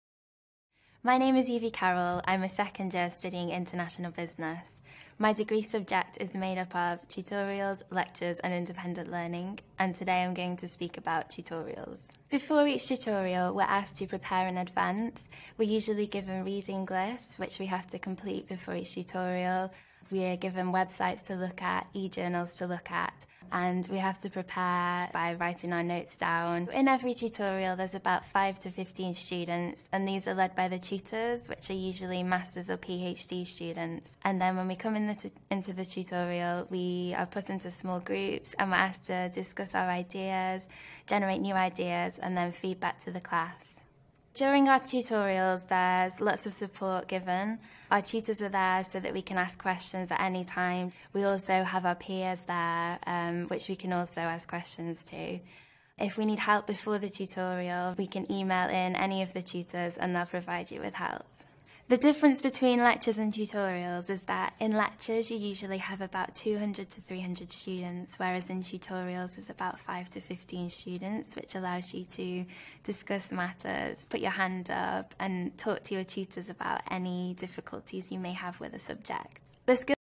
Panda PTE RL Retell Lecture-Tutorial真題機經
参考文本:
My name is Evie Carroll, I am a second-year sitting in International Business. My degree subject is made up of tutorials, lectures, and independent learning. And today I am going to speak about tutorials. Before each tutorial, we are asked to prepare in advance. We are usually given reading list which we have to complete before each tutorial, we are given websites to look at, e-journals to look at, and we have to prepare by writing our notes down. In every tutorial, there is about five to fifteen students, and led by the tutors, which are usually masters or PHD students. And then when we come into the tutorial, we are put into small groups, and we are asked to discuss our ideas, generate new ideas, and then feedback to the class. During our tutorials, there’s lots of support given, our tutors are there so that we can ask questions at any time. We also have our peers there which we can also ask questions to. If we need help before the tutorial, we can email in any of the tutors and they’ll provide you with help. The difference between lectures and tutorials is that in lectures, you usually have about 200 to 300 students; whereas in tutorials, there is about 5 to 15 students which allows you to discuss matters, put you hand up, and talk to your tutors about any difficulties you may have with the subject.
更多真題機經: Panda PTE